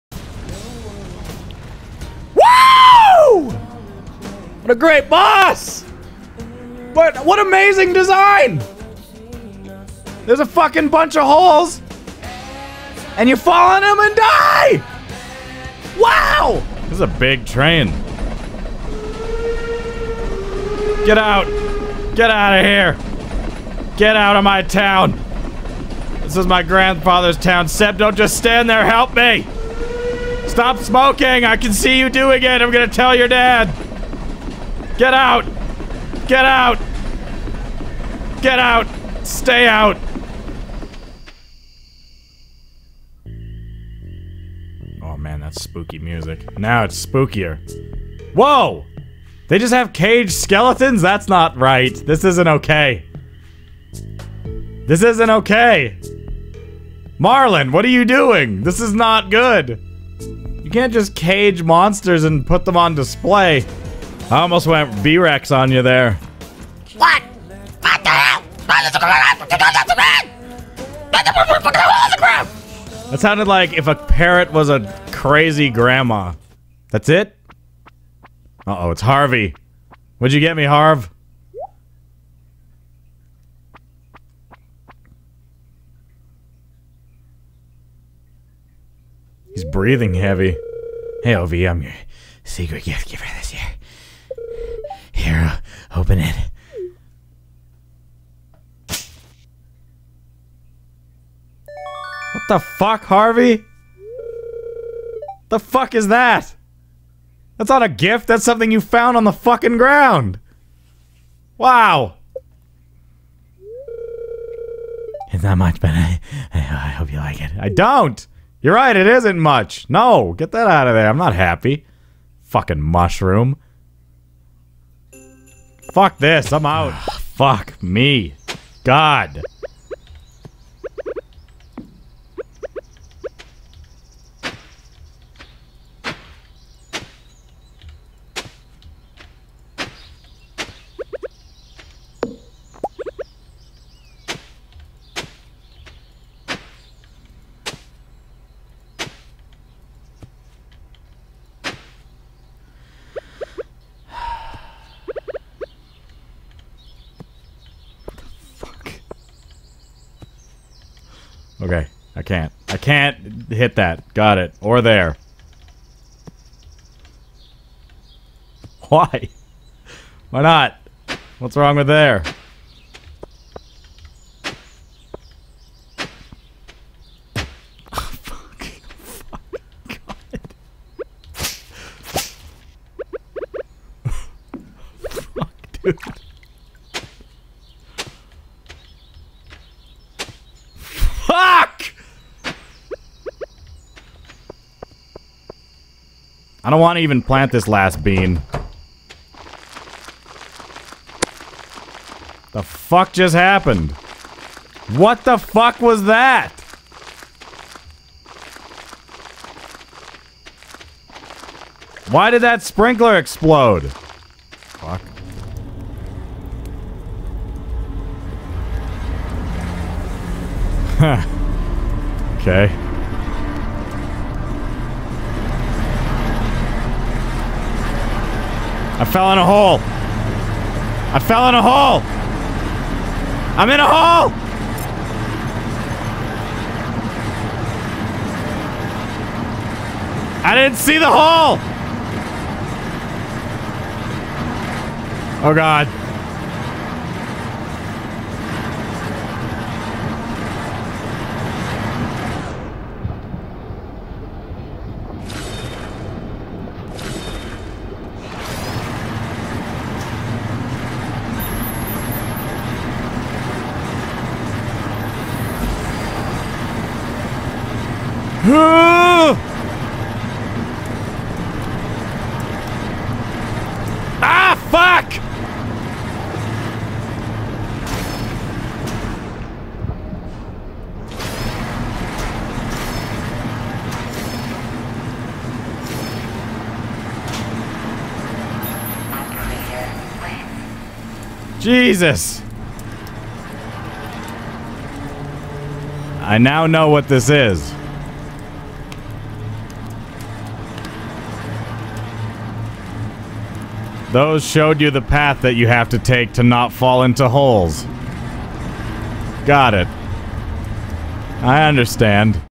The World's Shortest Arms | Old Vulpine Highlight Reel
Fantastic boss design and high pitched frustration. What more could you ask for?

All the highlights from this video are from Old Vulpine's Twitch livestreams.